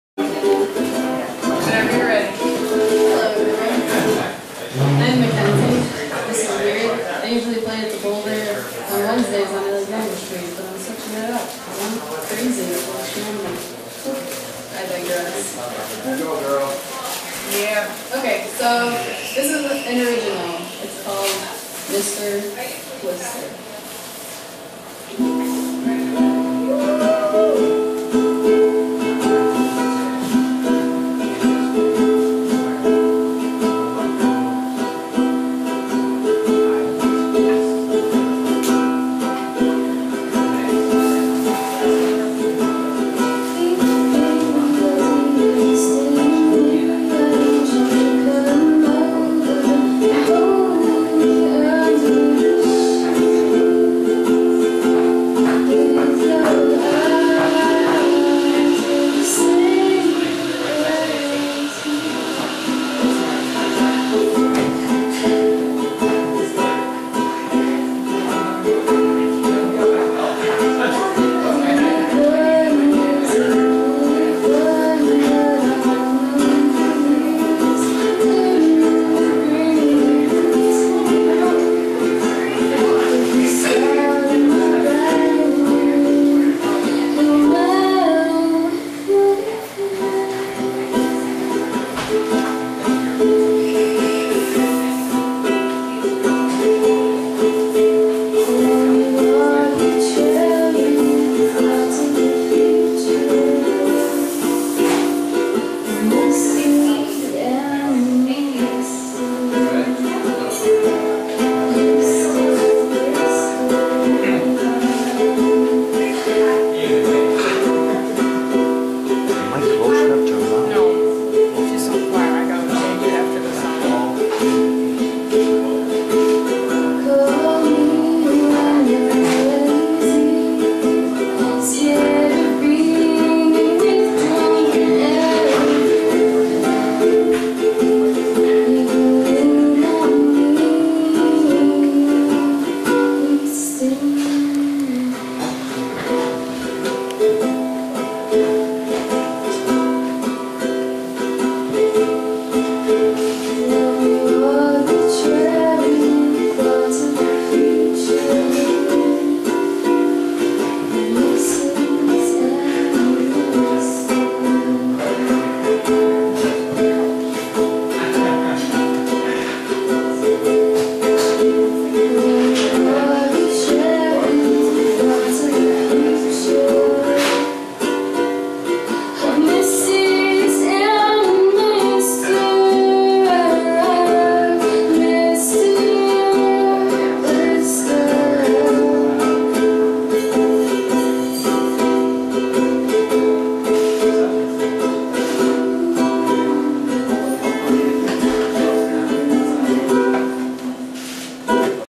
boulder house 1
the Boulder House on Park Ave,in Rochester NY, open mic  enjoy     video by Paul Refici